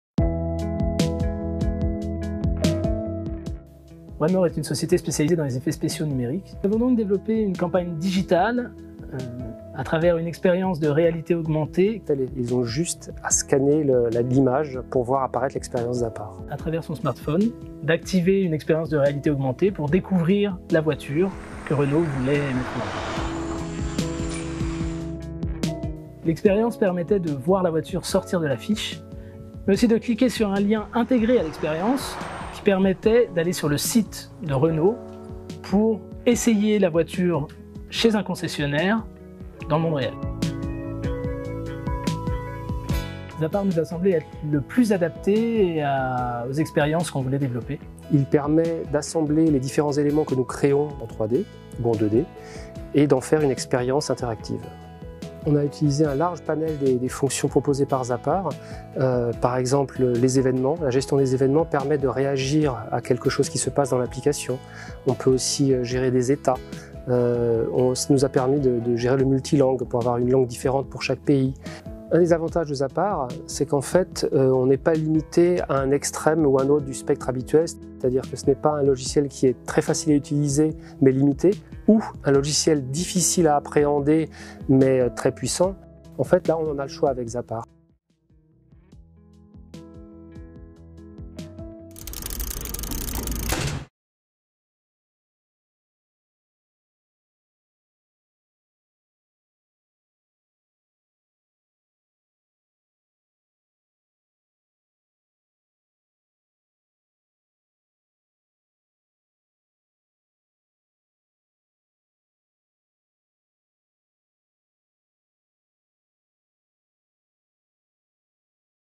Agence One More Montréal - Renault Kadjar pub concessionnaire le plus proche pour rendez-vous essai
Agence One More Montréal - Renault Kadjar pub trouvant automatiquement par géolocalisation le concessionnaire Renault le plus proche pour prendre en direct un rendez-vous pour faire un essai du modèle présenté.
Publicité déclenchée au départ d'un prospectus boites aux lettres